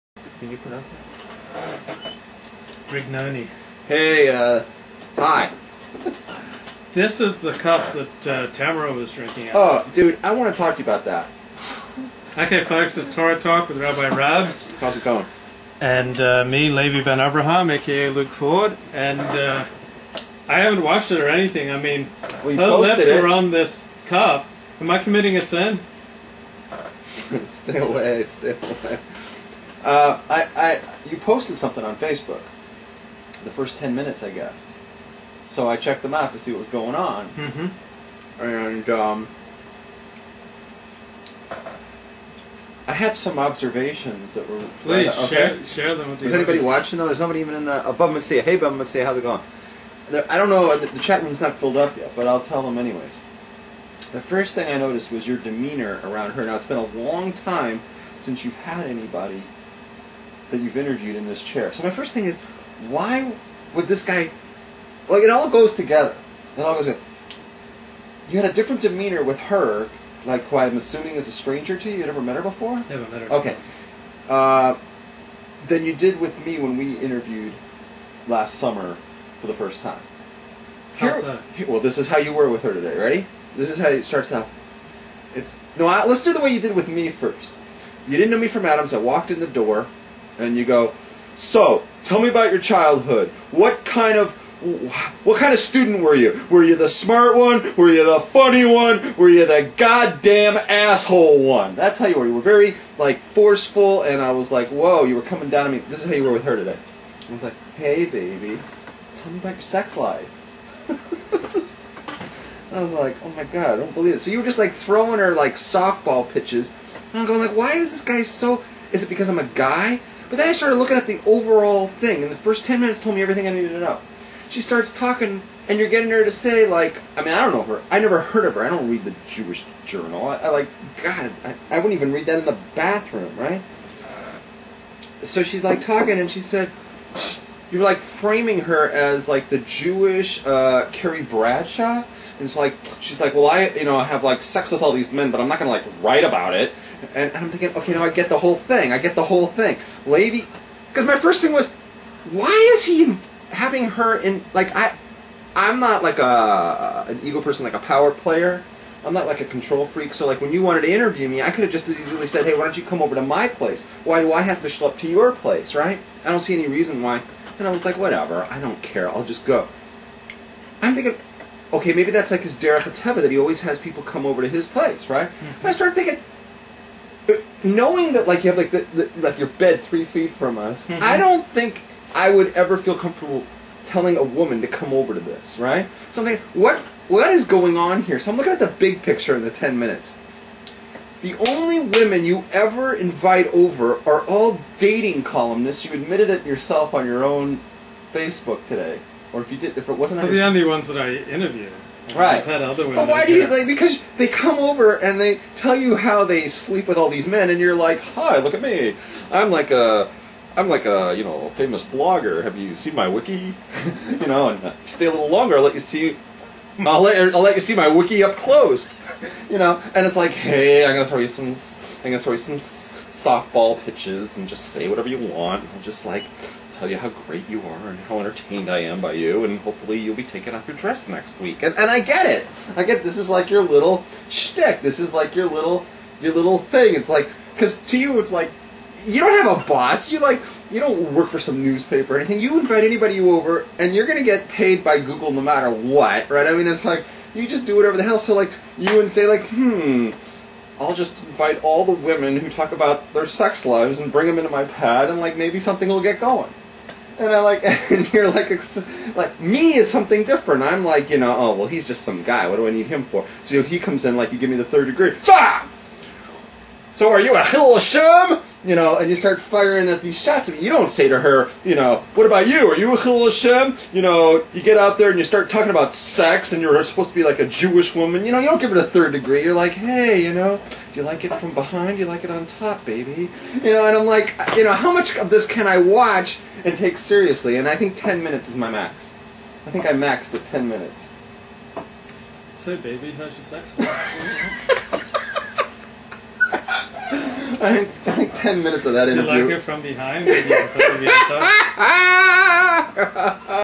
Luke Ford, Rabbs On Interviewing Women
Luke Ford, Rabbi Rabbs discuss an interview with a Jewish Journal dating columnist. Tattletales. Do I talk to women differently than I talk men? Torah portion Shemot.